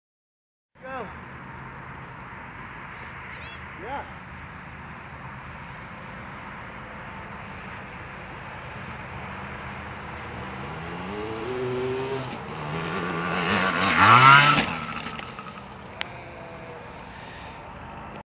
Austin jumping our canal on his dirt bike
Austin jumping the canal on his dirt bike.